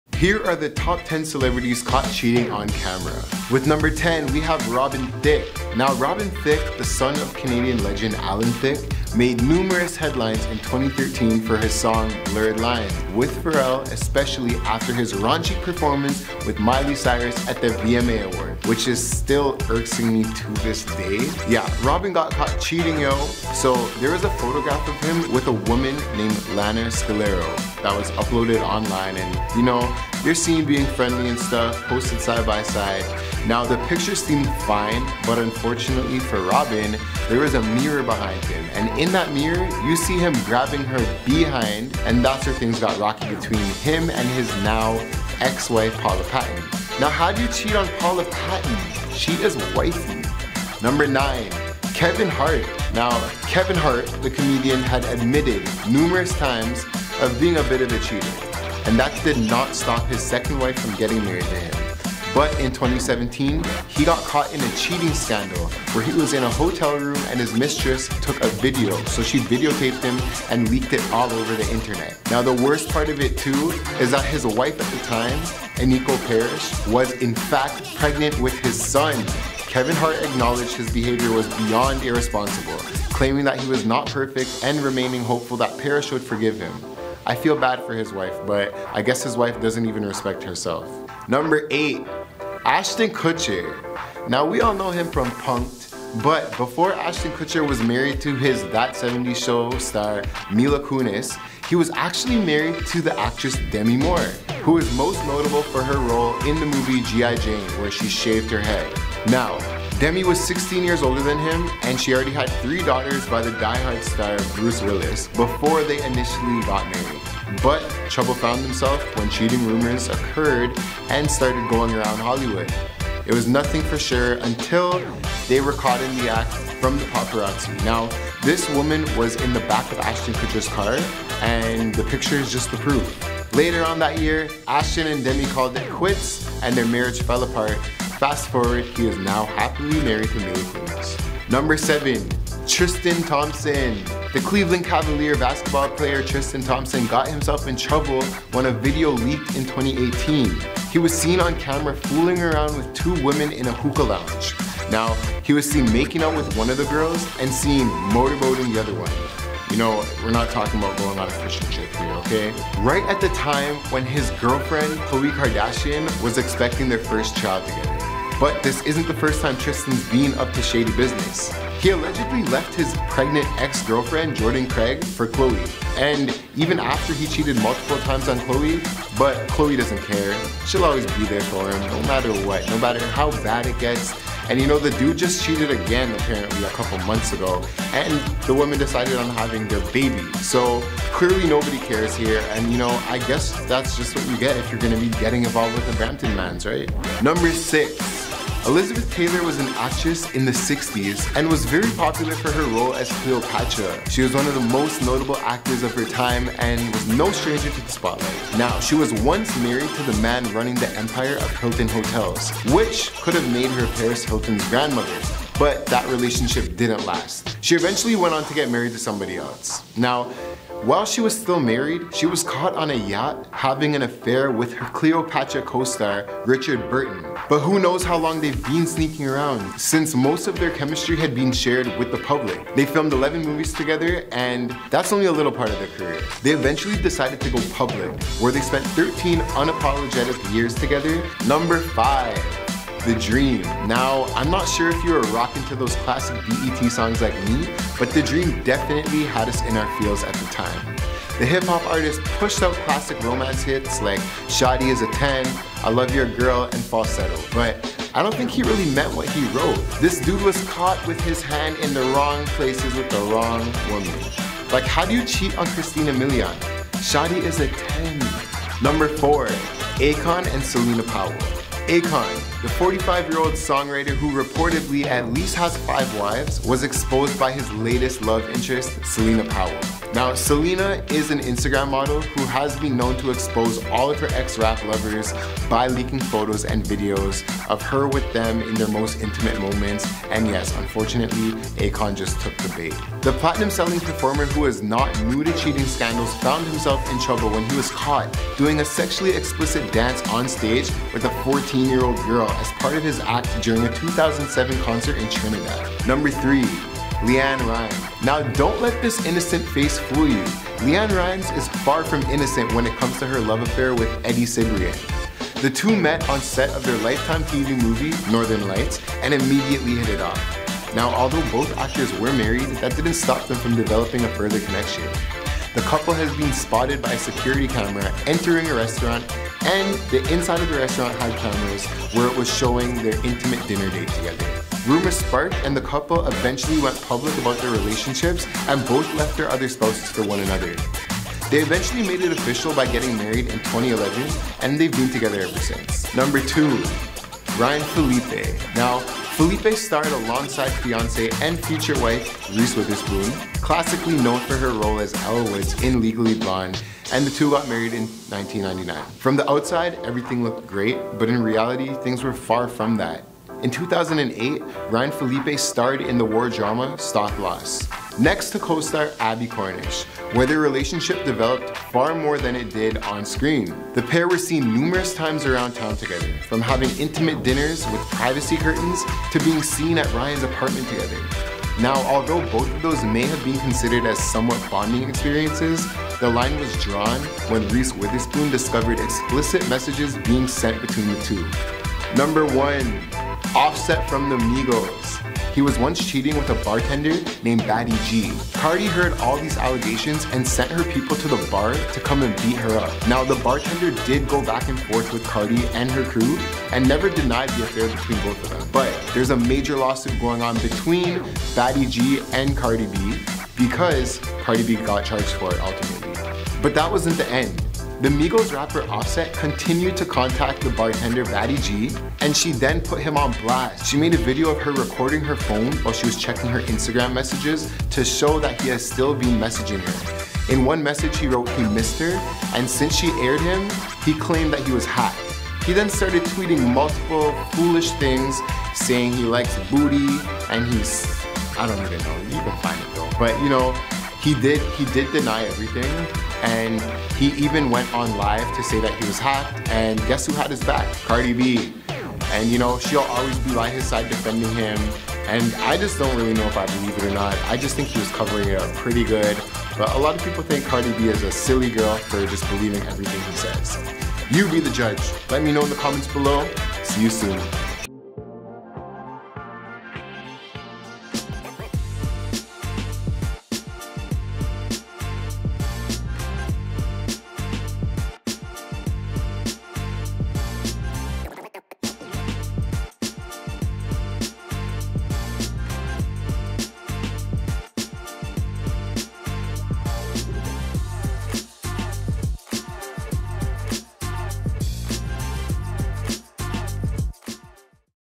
10 A-Listers Caught Cheating On Camera
10 A-Listers Caught Cheating On Camera

In this video, we're sharing 10 A-Listers who have been caught cheating on camera! From Tiger Woods to Justin Bieber, these stars have all been caught cheating on their significant others. Whether it's with another person or their phone, these celebrities have all been caught in some pretty scandalous situations.